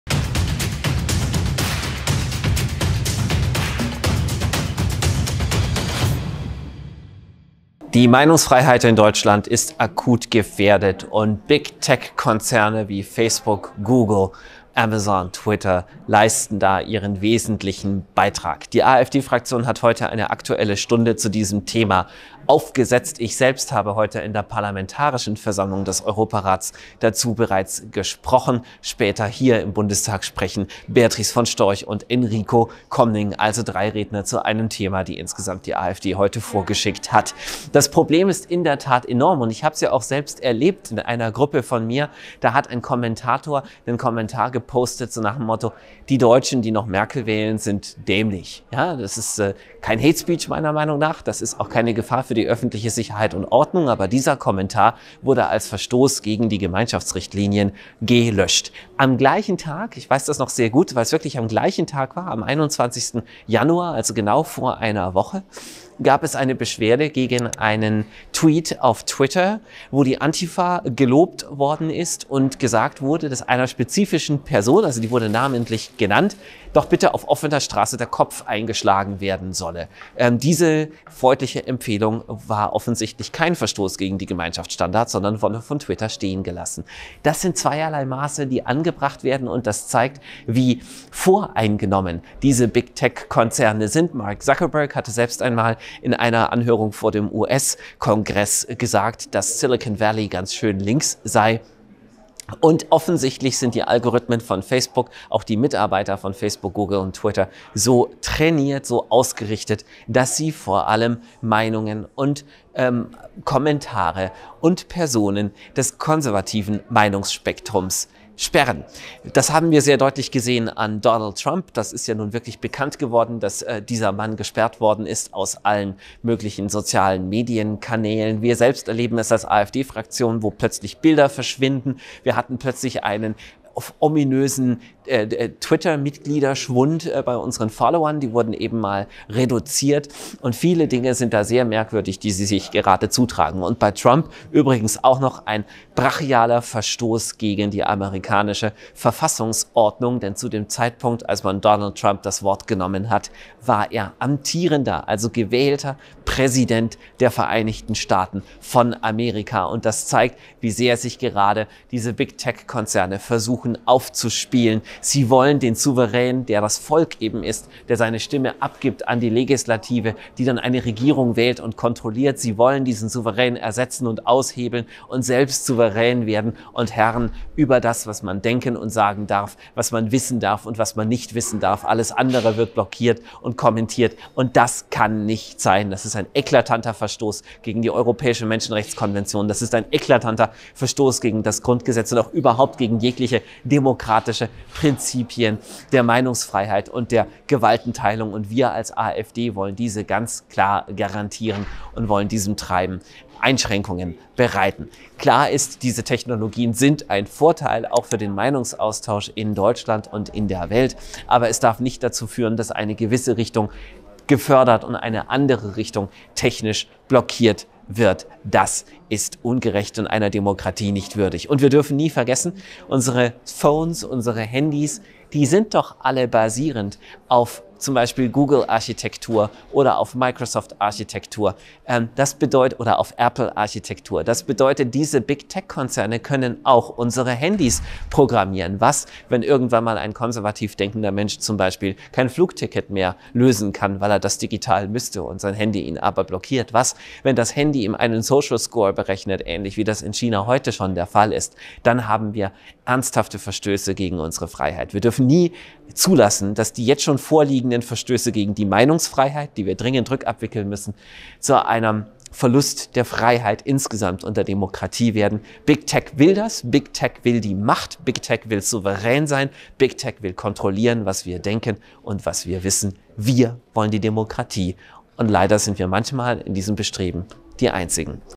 Meinungsdiktatur und Internetzensur nehmen zu! - Die Macht der Big Tech Konzerne
Heute habe ich in der PVER zum Thema Meinungsfreiheit und Internetzensur gesprochen. Ebenso gab es zu dem Thema eine aktuelle Stunde im Bundestag. Wir dürfen den Big-Tech Konzernen nicht die Meinungshoheit überlassen!